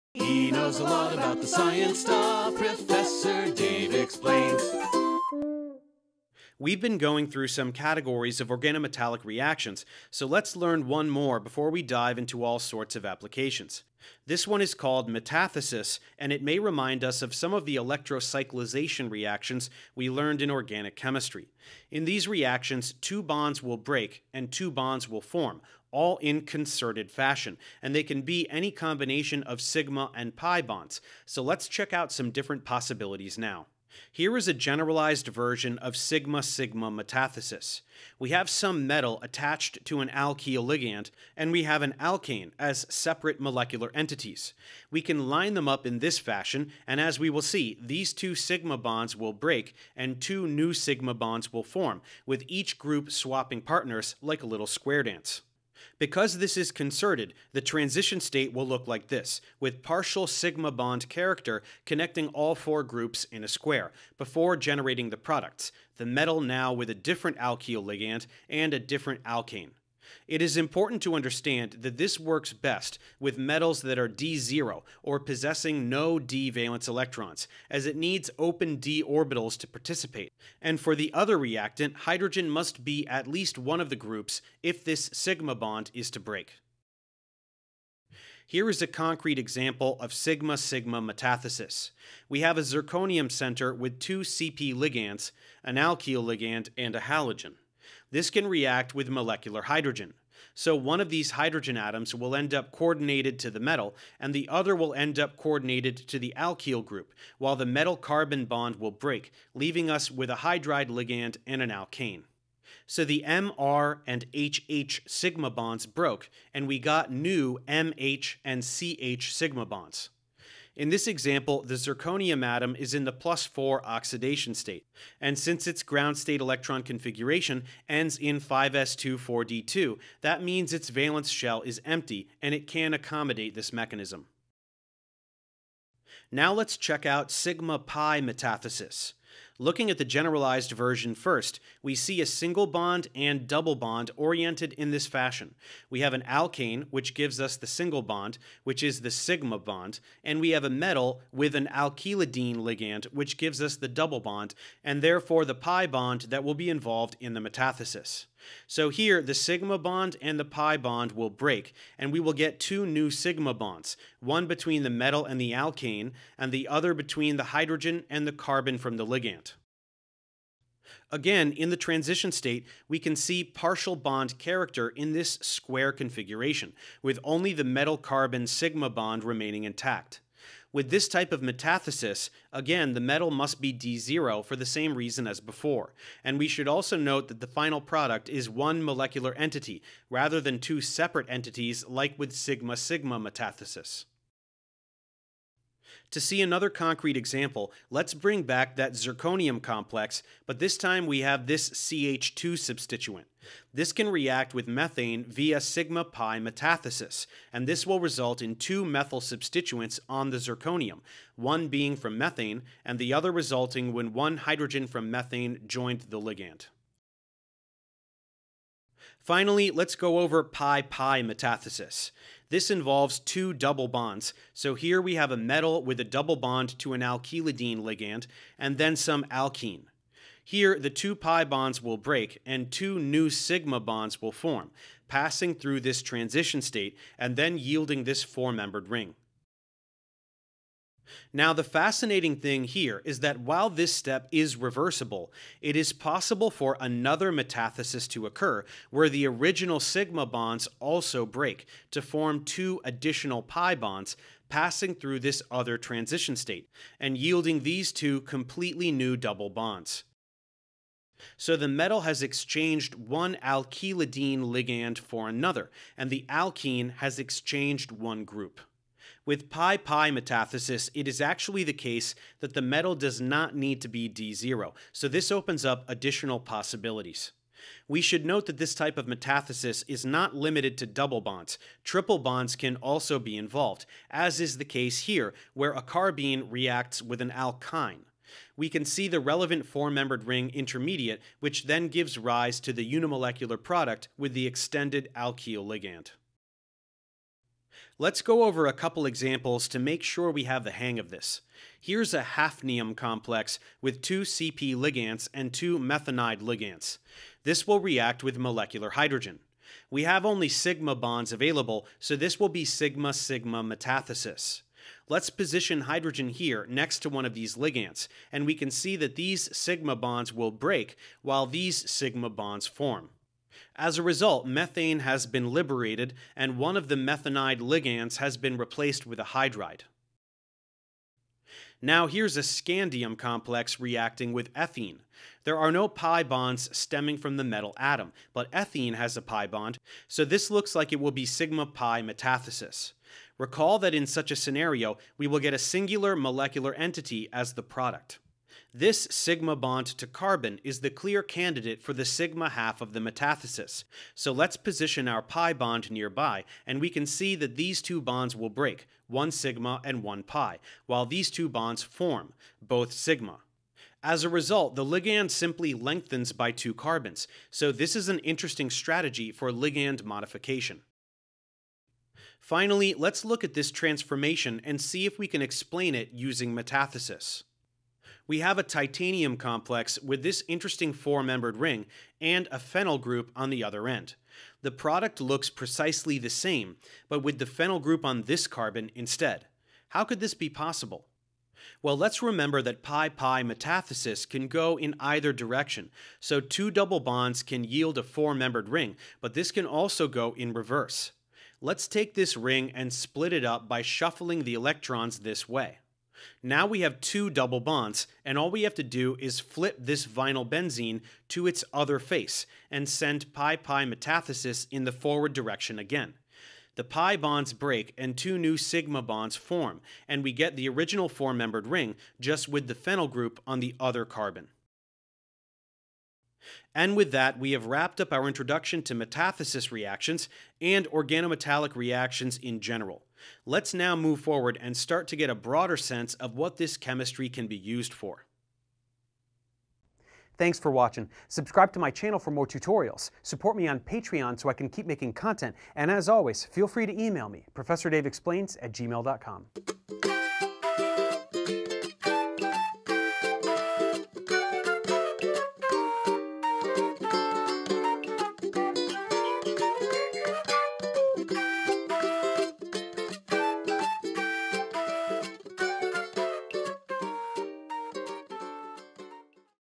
Organometallic Reactions Part 6: Metathesis Reactions
We have just one more type of organometallic reaction to go, and it's a really neat one! It's the metathesis. With these, two bonds will break and two bonds will form, in any combination of sigma and pi bonds. These can accomplish some really fascinating transformations, so let's see how these work now.